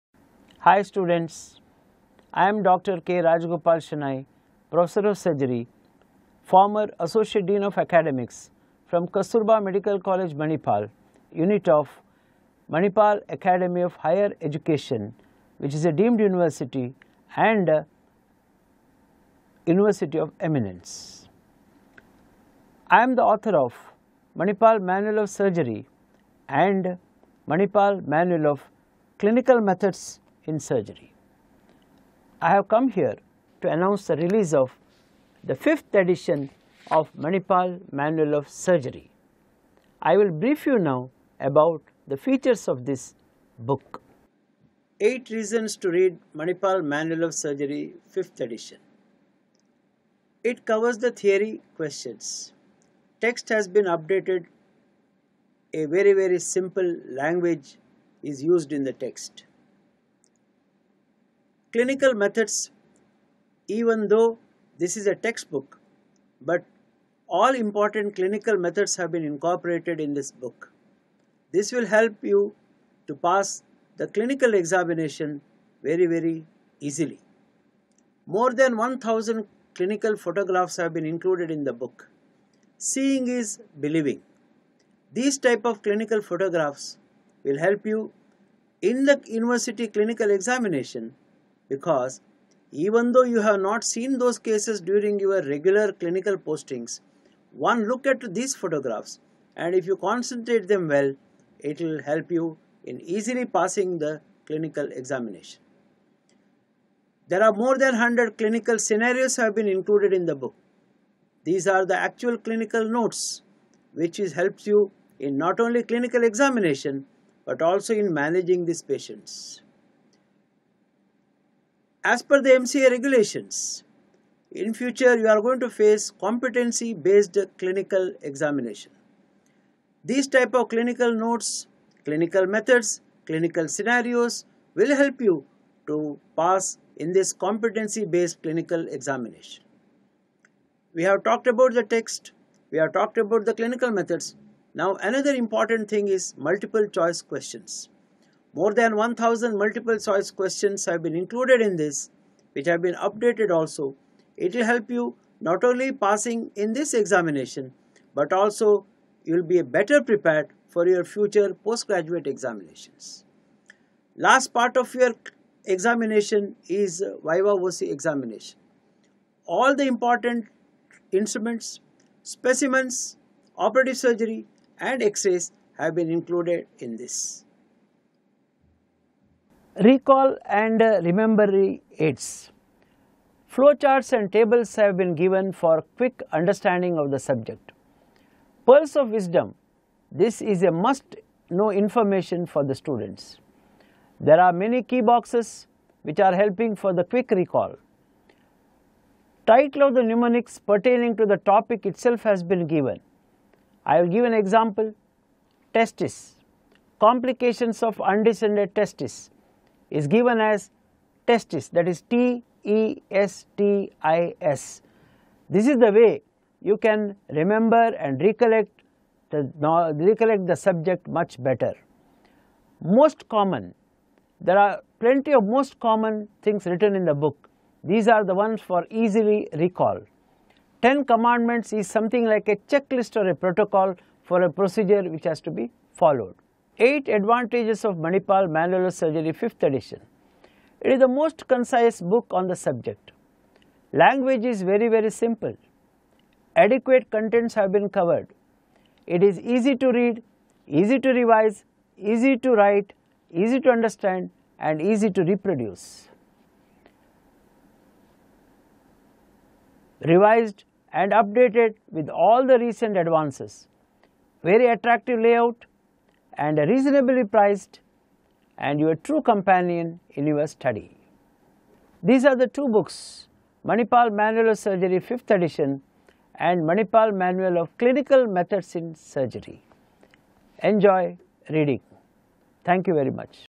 Dr. Shenoy Manipal Manual of Surgery
Dr. Shenoy Manipal Manual of Surgery, Manipal Manual Of Clinical Methods In Surgery Differential Diagnosis And Clinical Discussion 1st Reprint
Additional information

Publisher:  CBS Publishers & Distributors
Binding Paperback
Size: 23×36/8 trimmed 216×280mm
Year:  2019
Edition: 1/e
Pages: 552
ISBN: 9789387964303
Author: Rajgopal K Shenoy | Anitha Shenoy
Subject: Surgery

About Book: Manipal Manual of Clinical Methods in Surgery differential Diagnosis and Clinical Discussion is a unique book specially written for both undergraduate and postgraduate medical students to help them achieve distinctively in their clinical examinations. The limited time-frame available to undergraduate students makes it necessary for them to be able to read a good, comprehensive clinical book reliably: This manual fully meets just that requirement. The book presents the entire information necessary for undergraduate students in an easy-to-read format which will help the reader in understanding the facts with long-term retention. The frequently given clinical photographs, color graphics, flowcharts, tables, clinical boxes, and algorithms lucidly explain the facts discussed in the text. These will help even the postgraduate students of surgery and practicing surgeons.

The highlighting features of the Manual
Covers all clinical cases the students see in the general surgery clinical examination
Includes short and long cases
Colorful illustrations of clinical methods
Clinical discussion about common cases such as how? why? what?
Discusses all aspects of differential diagnosis
Clinical analysis on how to arrive at a diagnosis
Rich inserts like Clinical Case Capsules Clinical Wisdom
Provides key to secrets through clinical boxes
Problem-oriented discussions